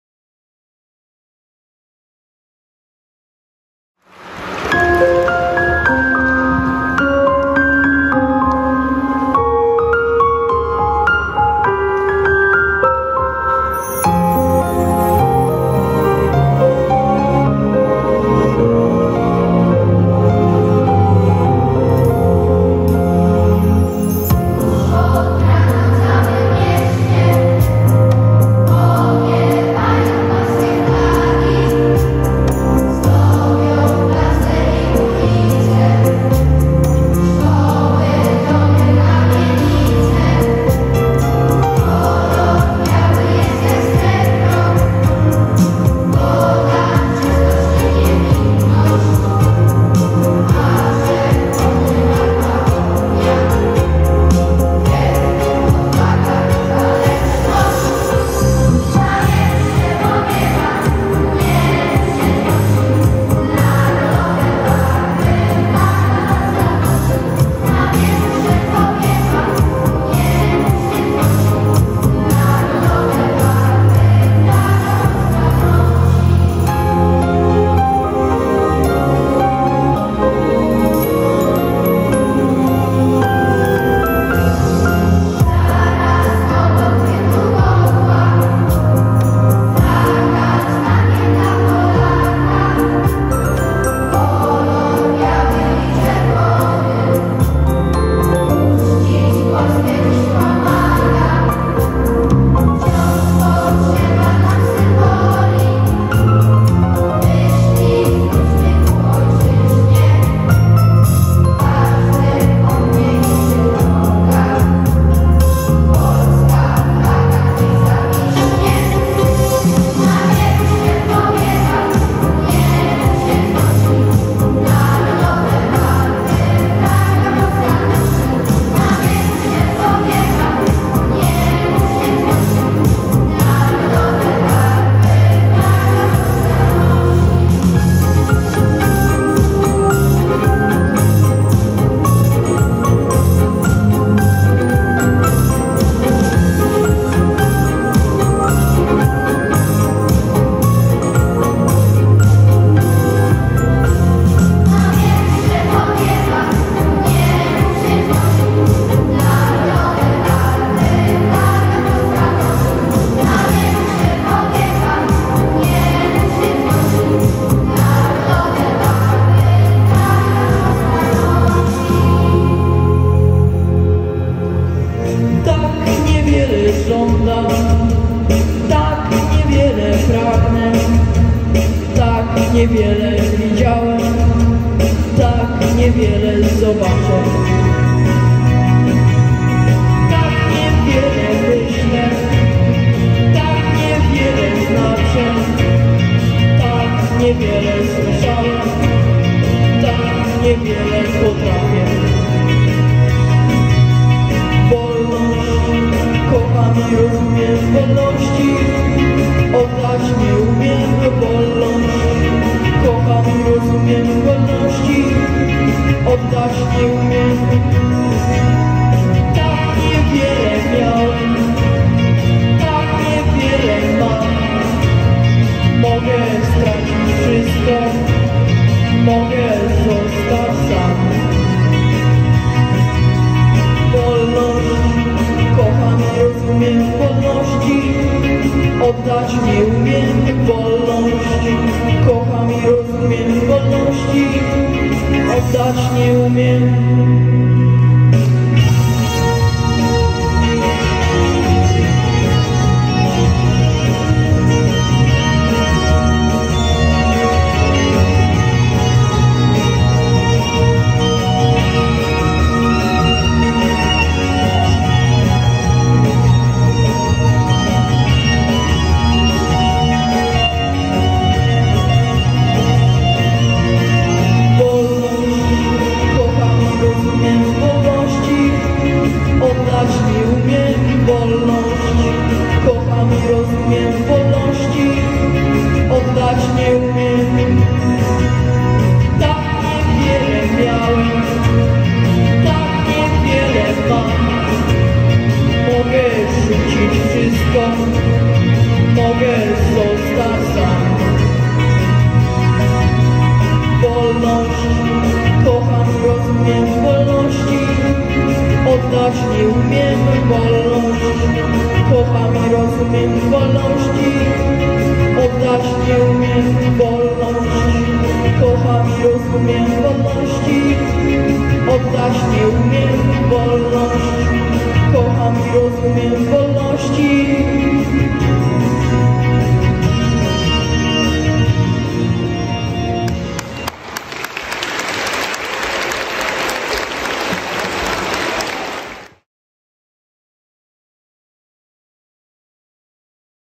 Społeczność „Piątki” uczciła 105. rocznicę odzyskania niepodległości przez Polskę
Szkoła Podstawowa nr 5 w Zambrowie
10 listopada 2023 roku